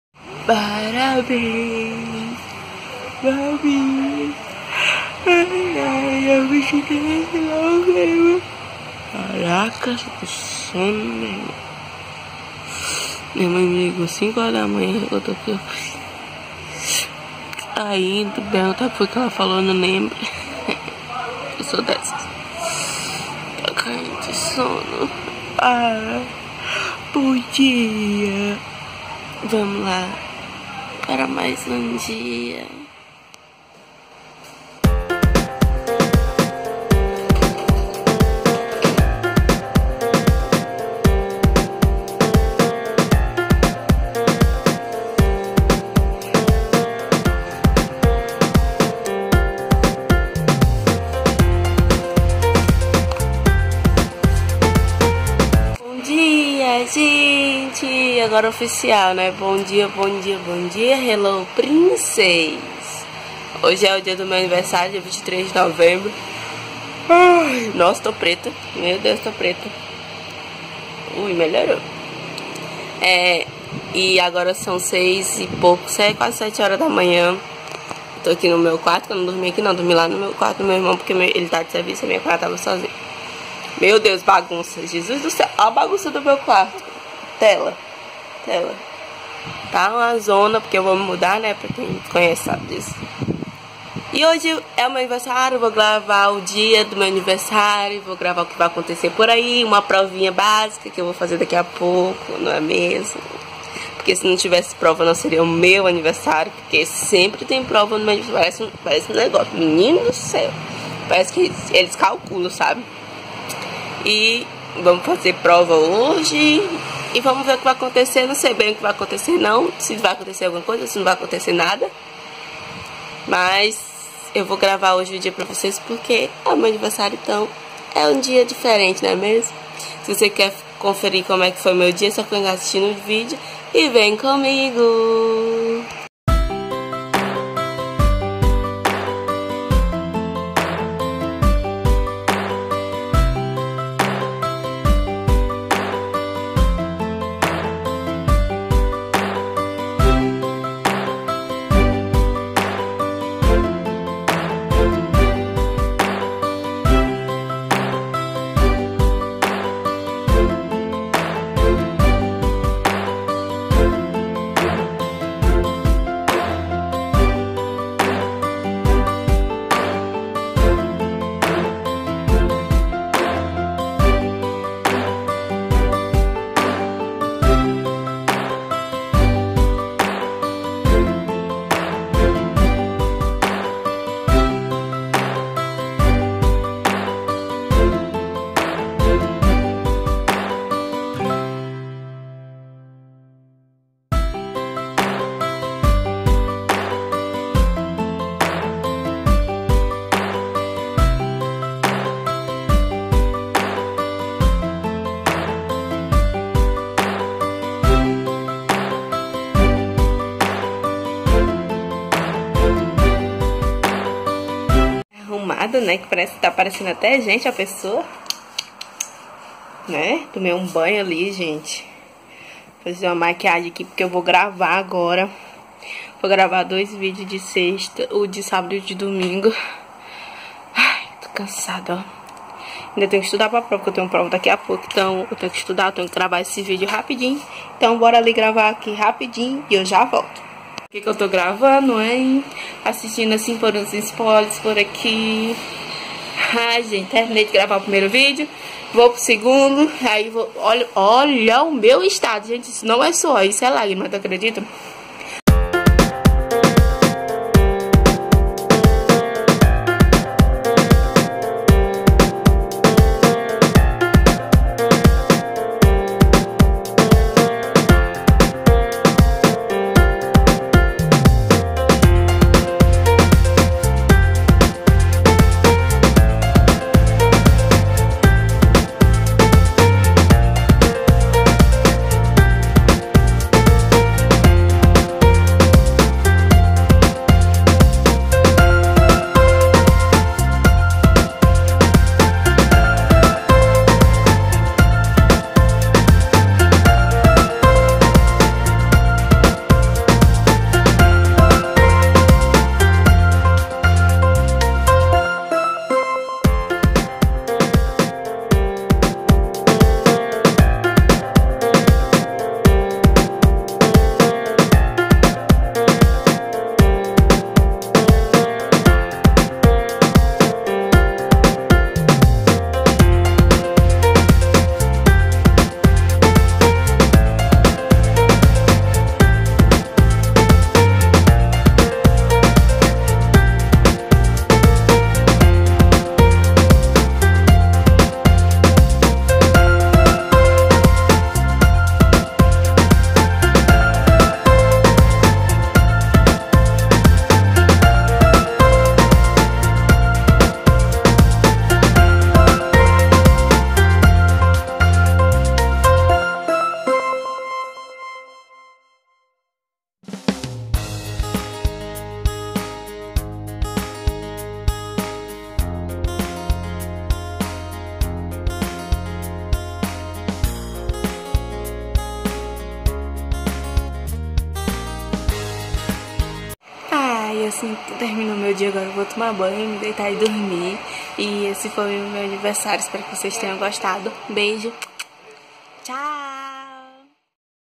Vlog: meu aniversário de 22 #24 por Thay Neri
Helloooo princeeesssss
Hoje eu trouxe um pouco do meu dia de niver, foi bem normal, mas ta valendo.